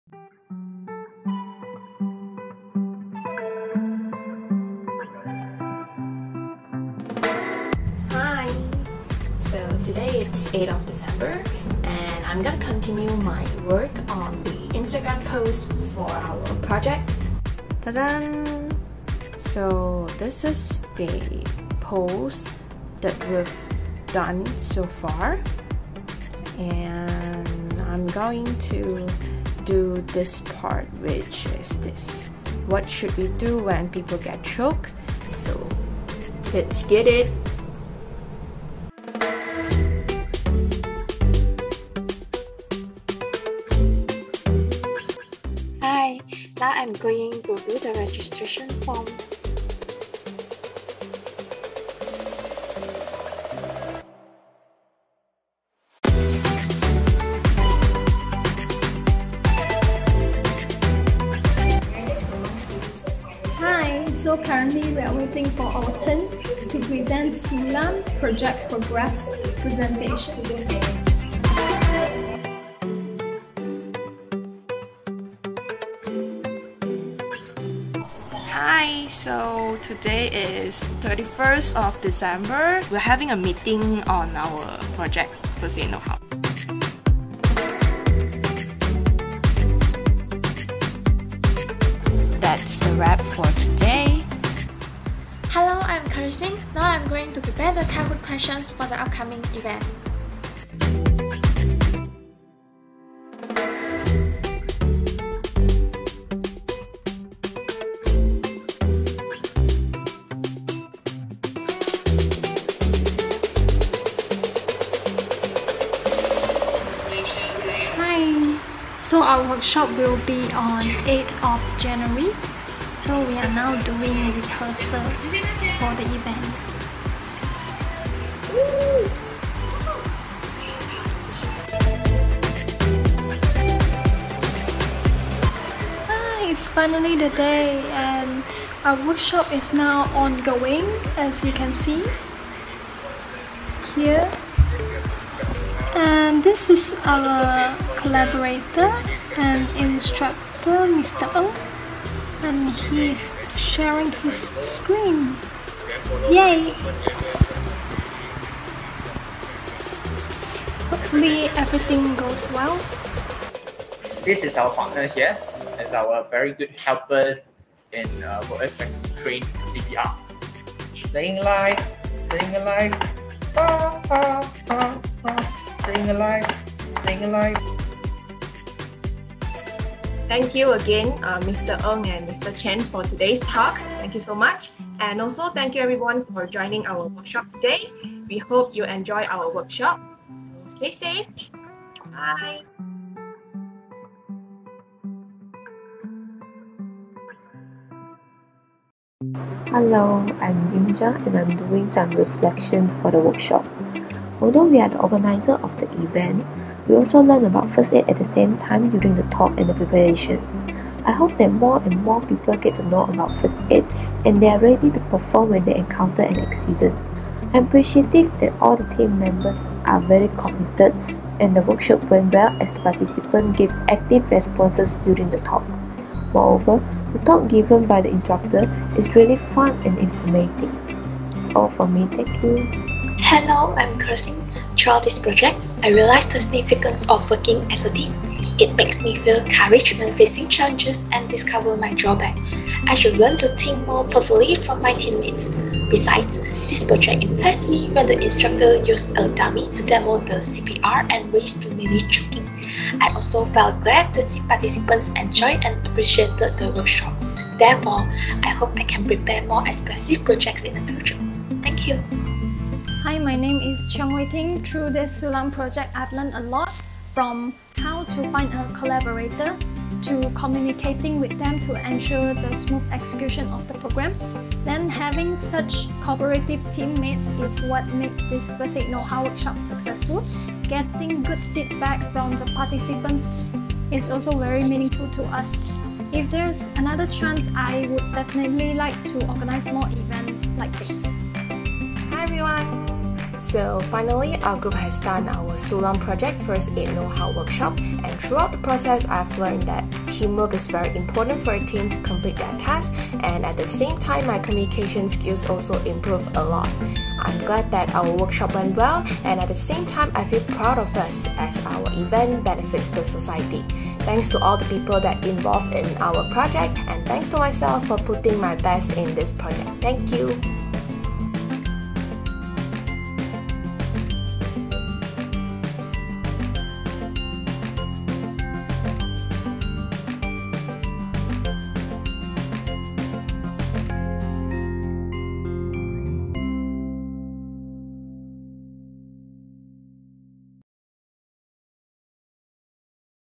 yuxuan's vlog #2
This is the video about the whole process of conducting the First Aid Know How workshop for our course WIG2001 Thinking and Communication Skills.

Thanks to my ummmazing members that always help out each other whenever there's need ✨✨
Hui Ting
Ker Xin
Jing Jia
Fernando

Recorded @ 2021.